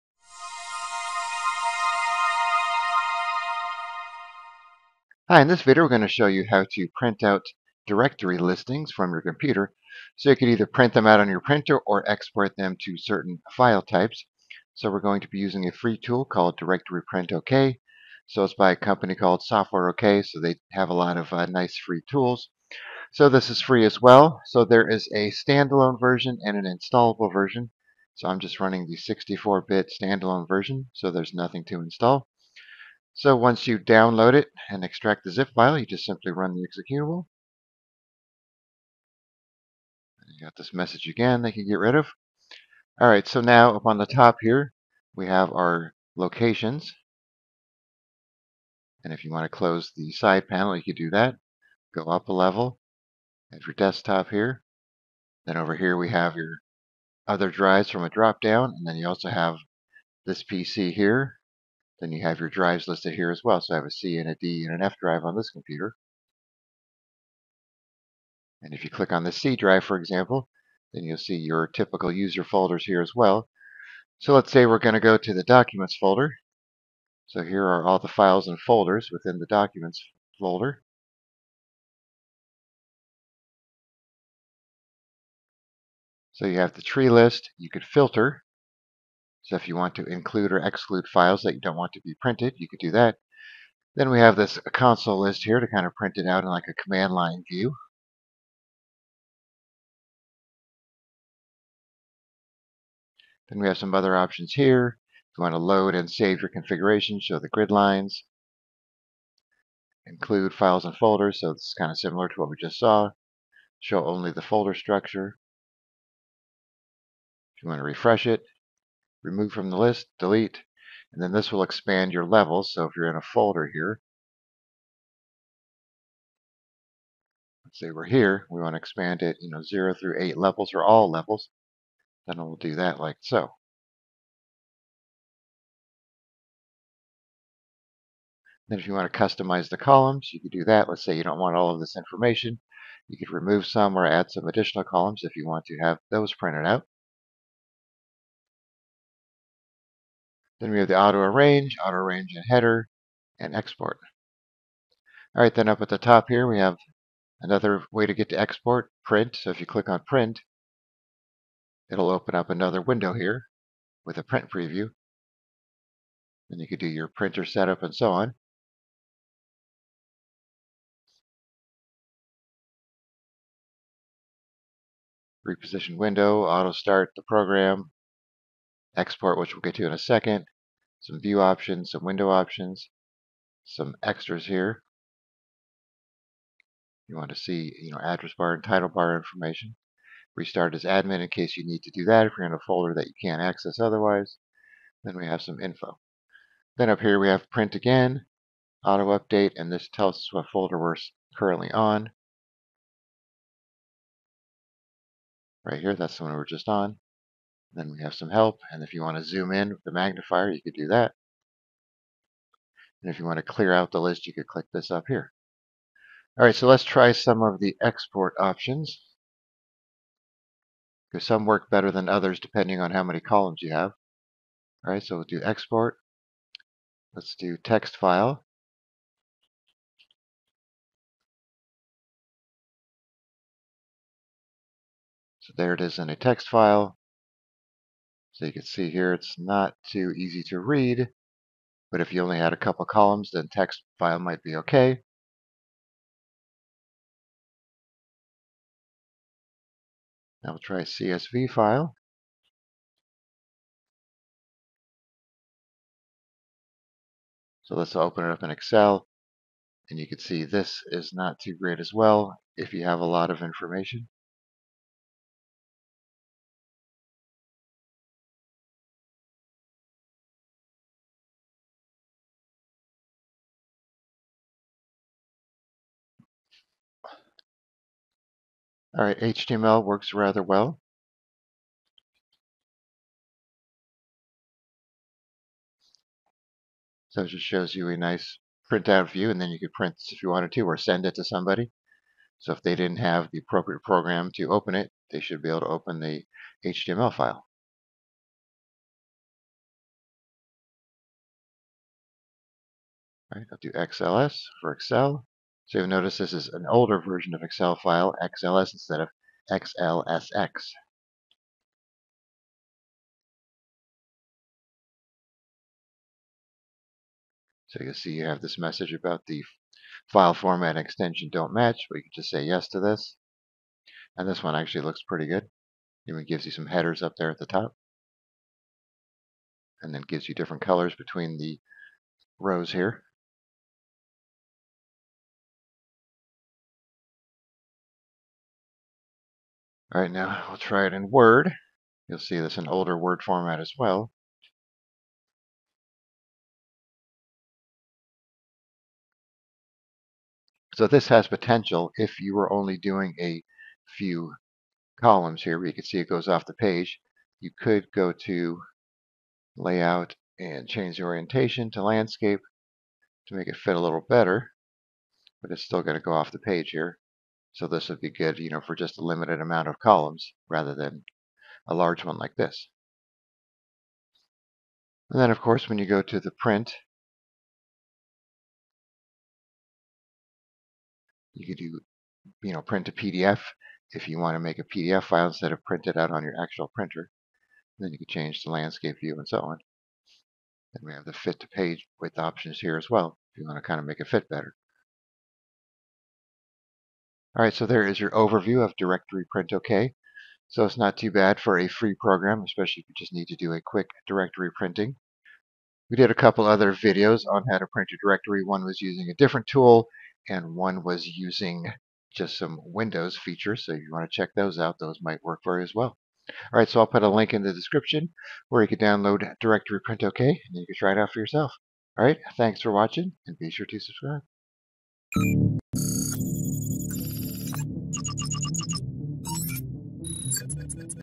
Print and Export Folder and File Directory Listing with DirPrintOK
If you need a quick and easy way to export or print out a listing of files and folders in a specific directory, then you can try the free DirPrintOK software. This tool will let you choose the directory and then customize what information will be printed. You can then print it to your printer or export the directory listing to a text file, HTML file, Word document, Excel spreadsheet, CSV file or PDF file.

Here is a link where you can download DirPrintOK.

Check out our book titled Windows 11 File Management Made Easy to take your file management skills to the next level!